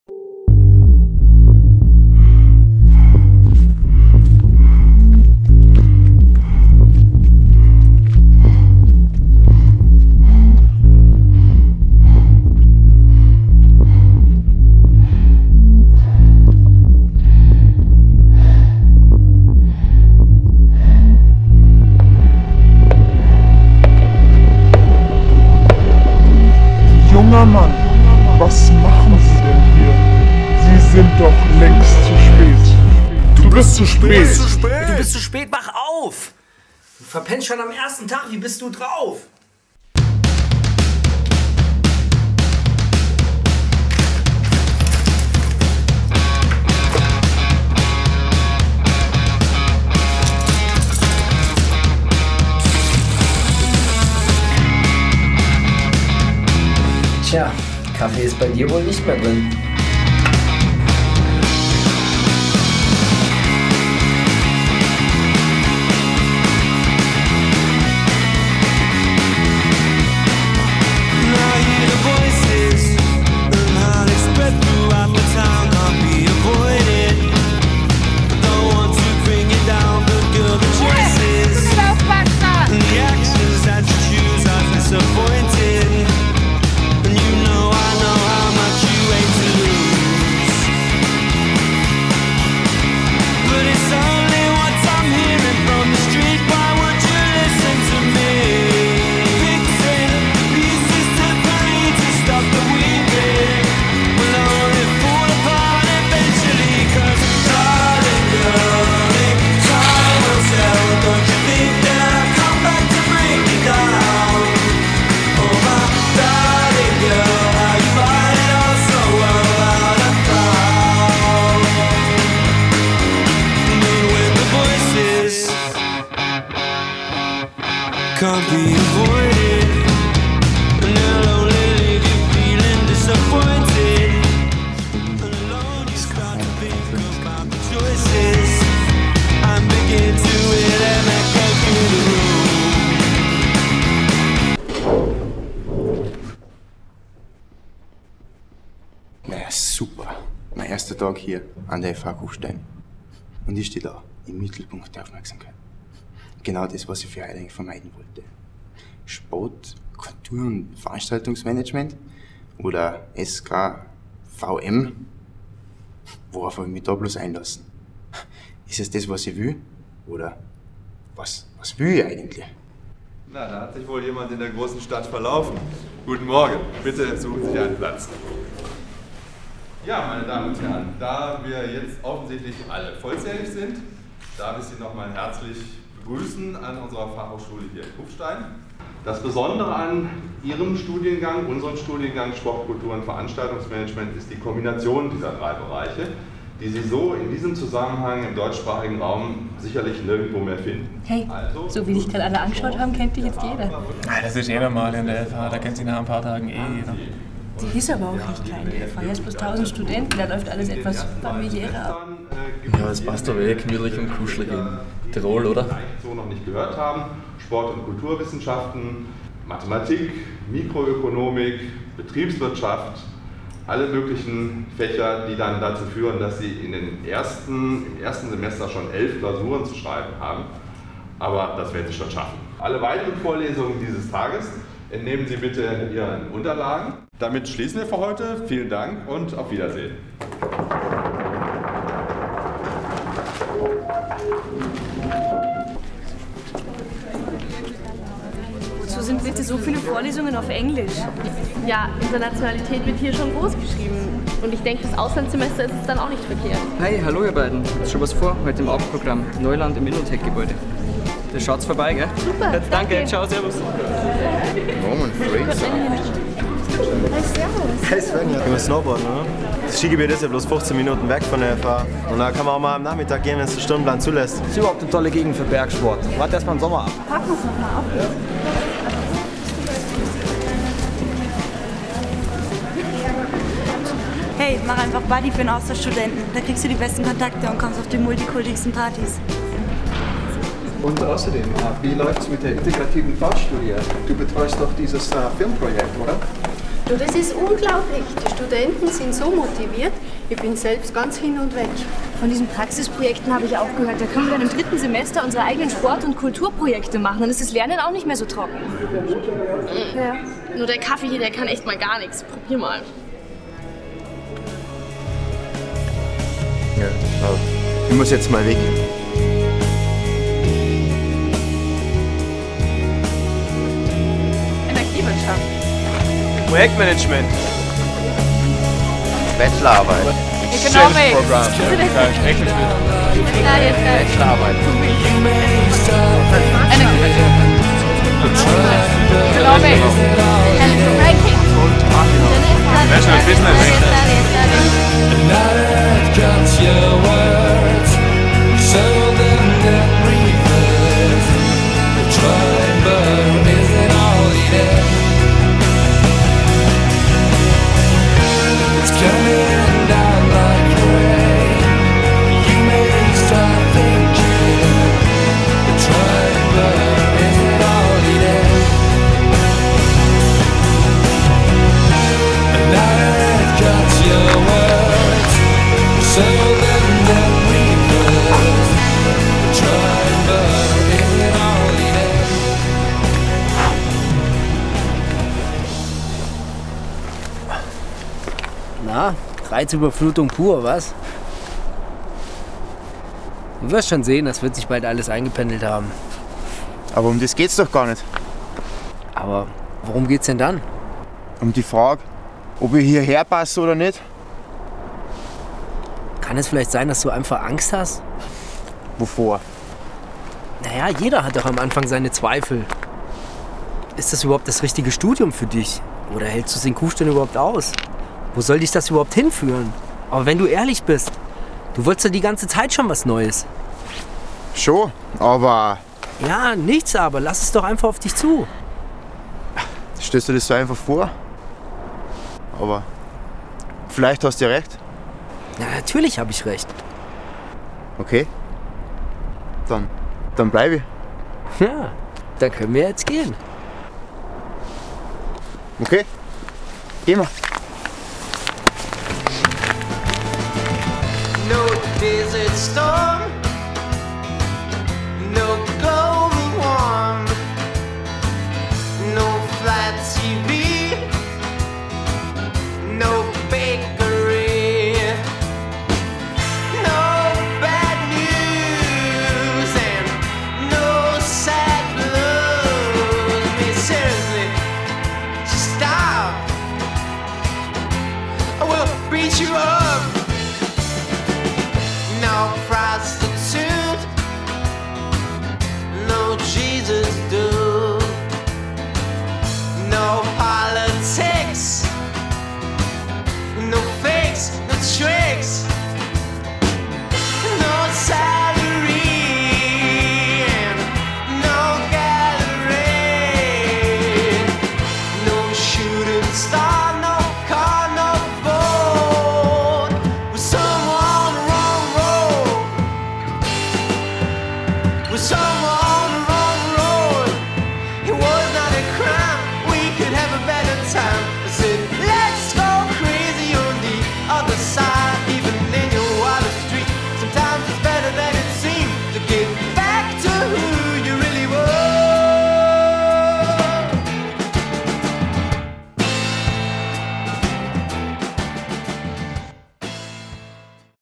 NEULAND
Ein Kurzfilm über den ersten Tag eines Studenten an der Hochschule Kufstein - erfahren, erleben, entscheiden.